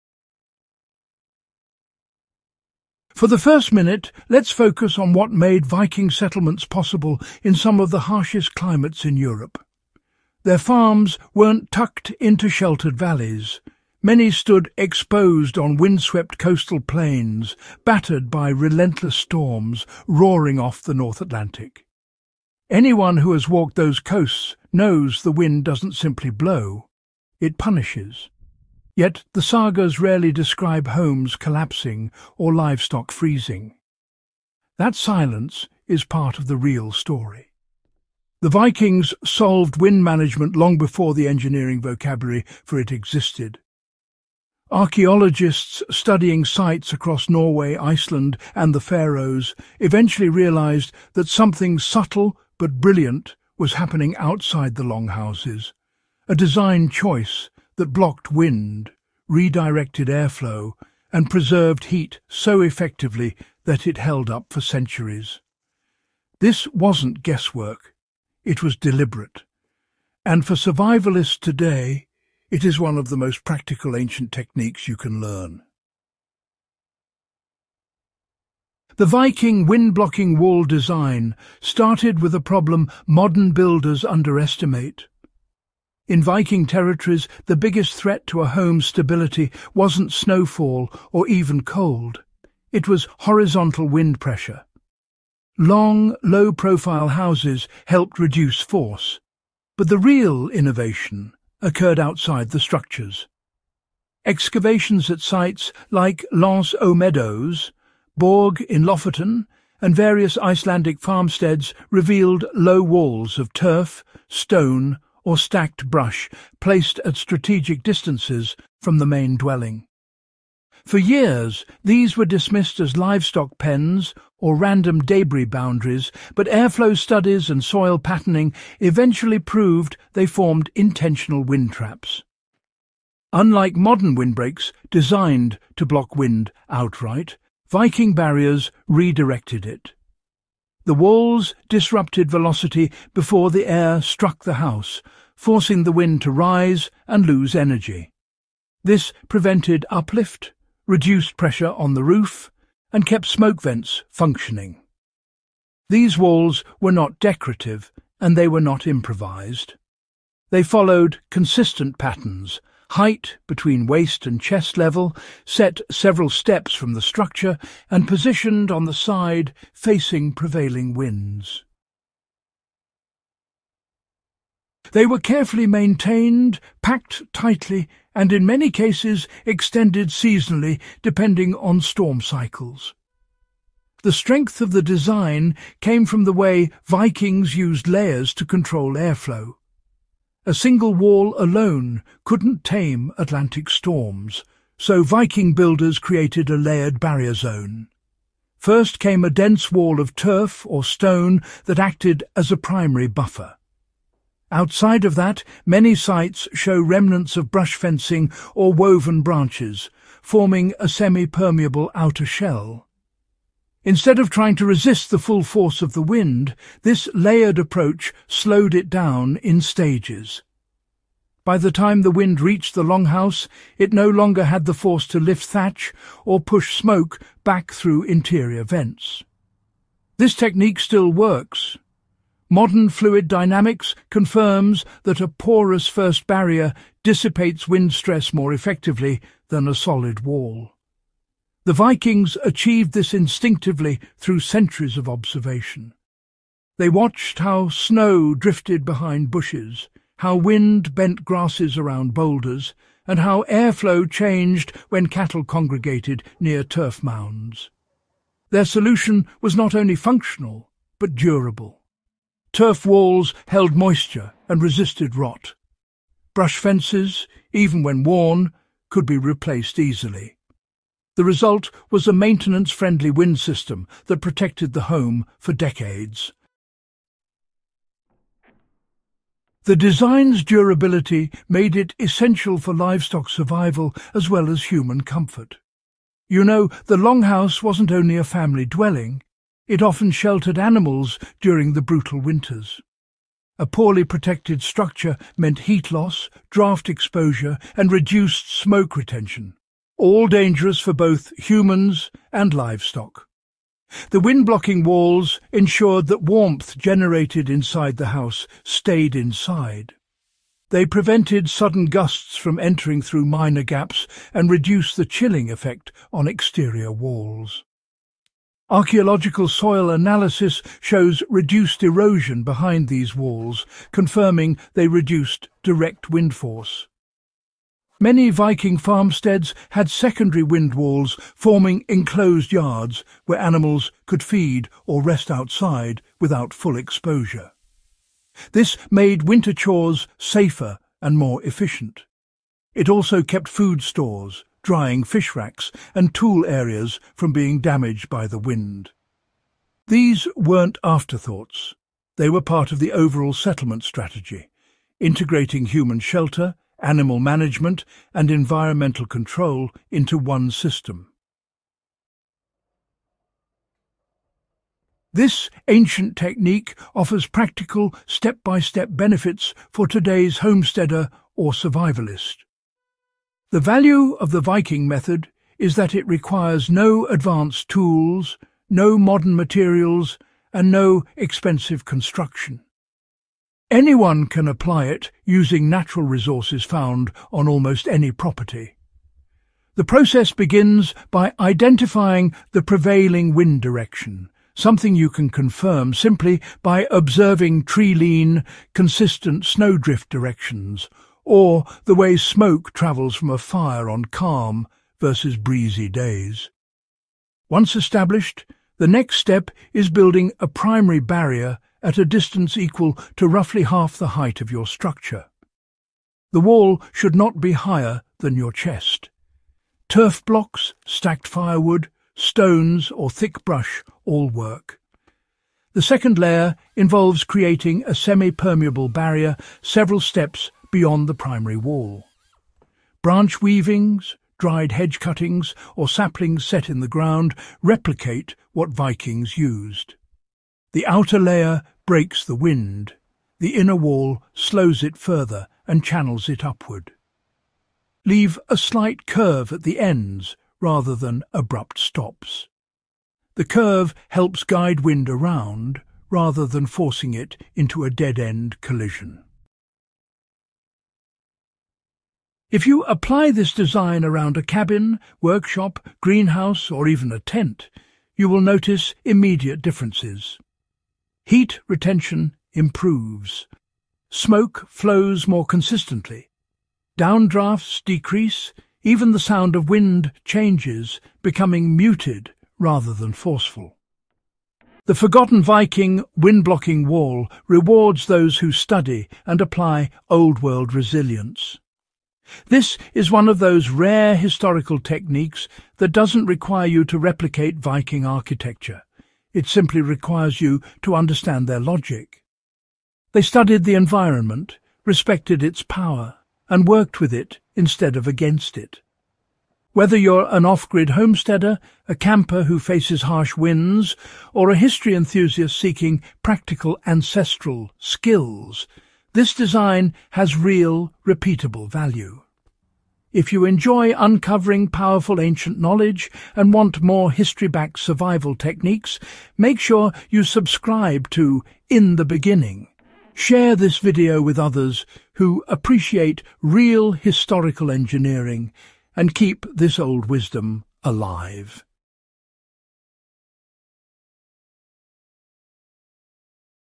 The Viking Wind Blocking Wall Design Hidden for Centuries
The Viking wind-blocking wall design used angled beams, turf layering, and airflow redirection to create storm-proof outdoor zones. Builders hid this method for centuries because it made camps, drying racks, forges, and shelters nearly windless.
Packed with Viking engineering, wind barrier design, primitive architecture, Nordic timber tricks, ancient survival shelters, and stormproof building methods.
This video uses AI narration and does not impersonate any individual.